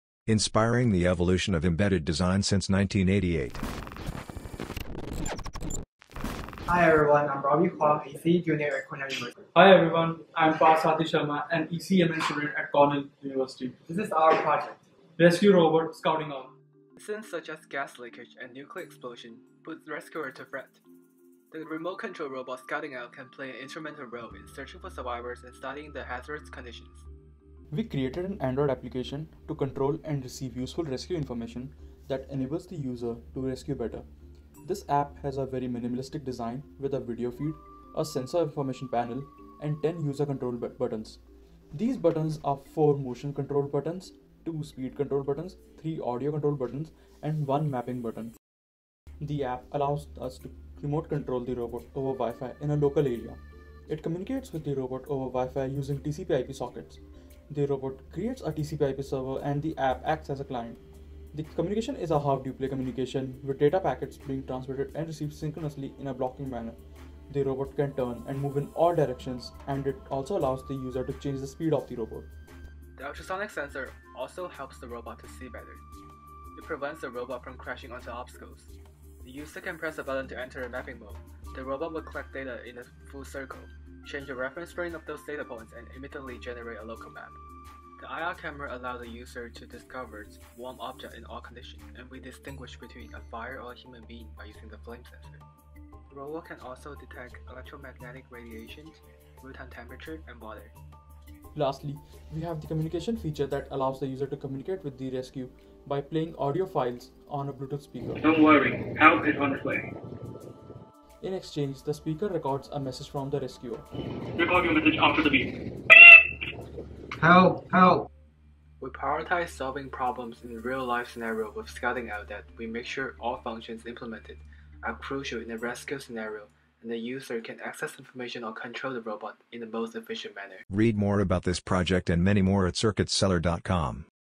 Raspberry Pi Rescue Robot - The Scouting Owl Engineering Project
Learn how these Cornell students built a Raspberry Pi 4-based robot that allows rescuers to detect and communicate with endangered individuals in an environment that is not yet suitable for in-person rescue LEARN MORE...

New Circuit Cellar Magazine comes out every month with new articles going live regularly

"In incidents such as fire, earthquake, flood, gas leakage or any environment that has potential threats to the life of a rescuer, a rescue robot can play an important role in searching for endangered individuals and studying hazardous conditions. "